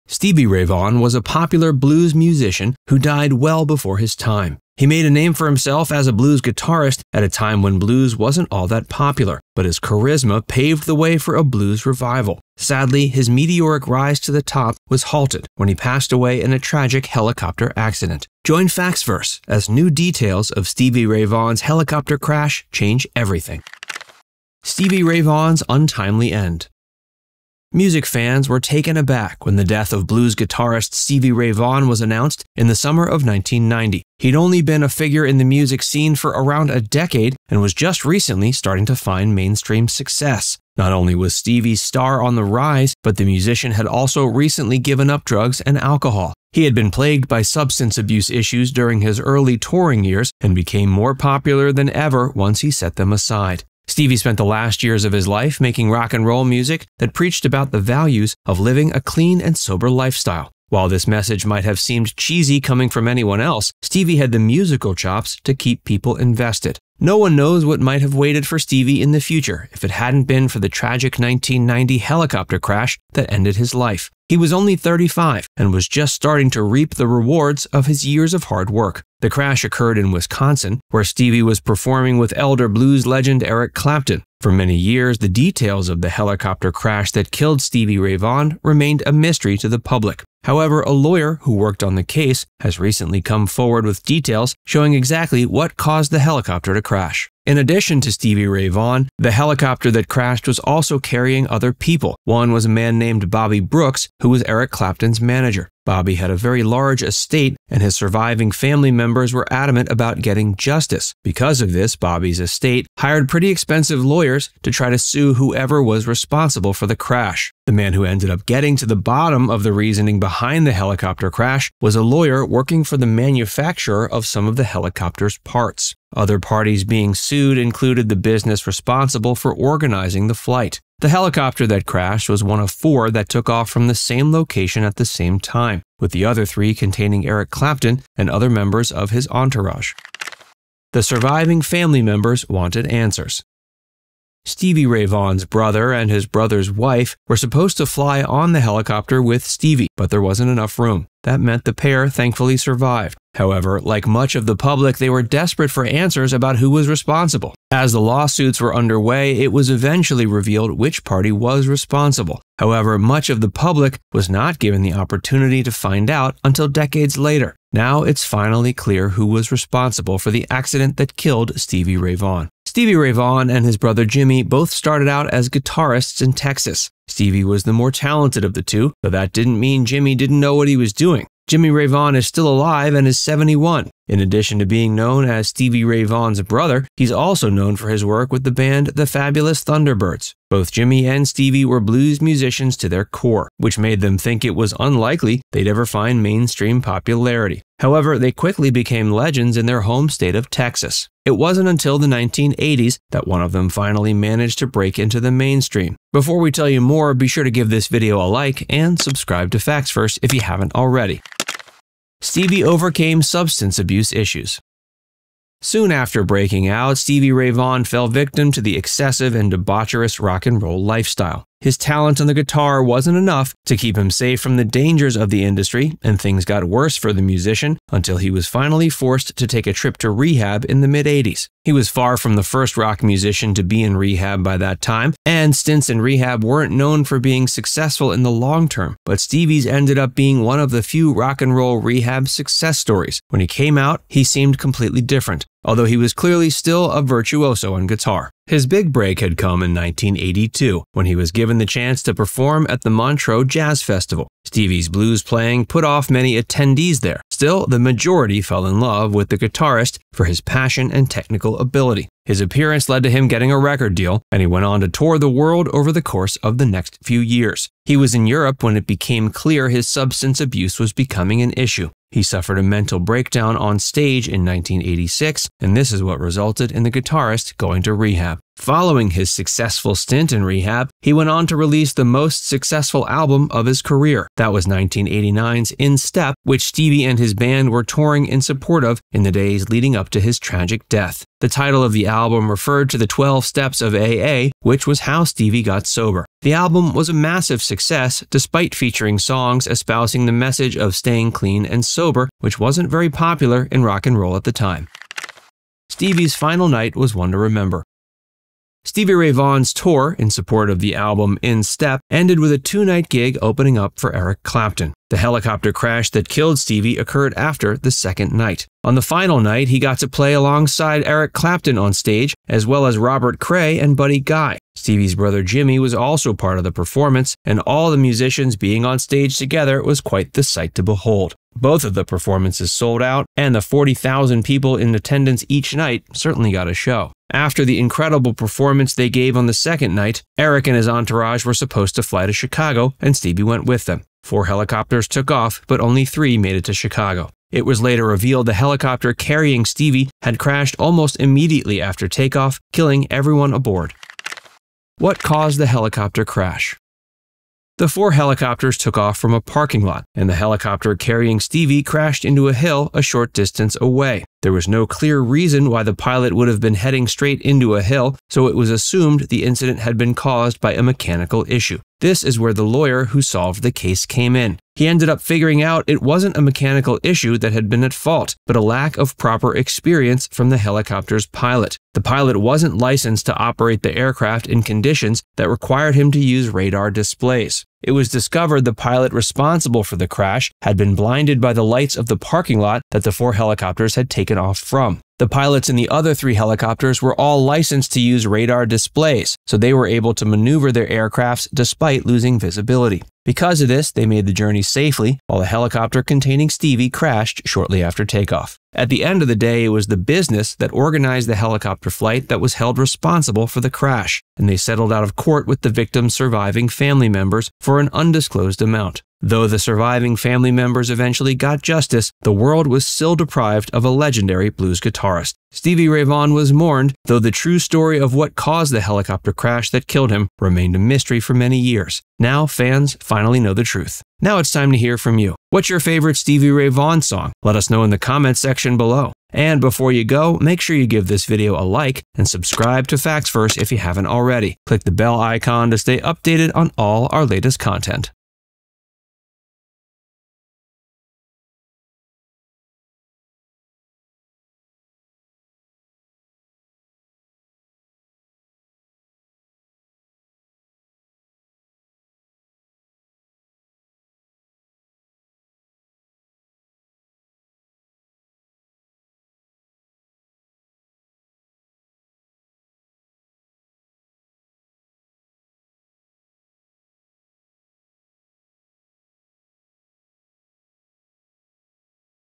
New Details of Stevie Ray Vaughan’s Helicopter Crash Change Everything
Did you know that Stevie Ray Vaughan’s final album was named after the Twelve Steps of AA, and that he got to perform alongside both Eric Clapton and Buddy Guy on the night of his death? Stevie Ray Vaughan was a musician that died well before his time. Stevie made a name for himself as a blues guitarist at a time when blues music wasn’t all that popular, but the star’s charisma allowed him to pave the way for a blues revival. Sadly, Stevie’s meteoric rise to the top was halted when he passed away in a tragic helicopter accident. For many years, the public was left guessing as to what had made the 1990 crash occurred. In 2020, a lawyer who worked on the case revealed the details of the crash to the public for the first time, finally answering the question of who was responsible for Stevie’s death.

00:00 - Intro
00:24 - Stevie Ray Vaughan’s Untimely End
02:21 - The Surviving Family Members Wanted Answers
03:32 - Stevie Overcame Substance-Abuse Issues
05:06 - Stevie’s Final Night Was One to Remember
05:55 - What Caused the Helicopter Crash?
07:14 - Outro

Stevie Ray Vaughan rose to prominence in his home state of Texas before eventually finding national recognition in the 1980s. In the mid-1980s, Stevie had a mental breakdown while touring Europe that was caused by substance abuse. He then went to rehab, and he emerged a changed man! Stevie found unprecedented mainstream success following this rehab stint, and he was at the top of the world when tragedy struck.

Stevie was just finishing off a tour in support of his latest album when his life was tragically cut short. Join Facts Verse as new details of Stevie Ray Vaughan’s helicopter crash change everything.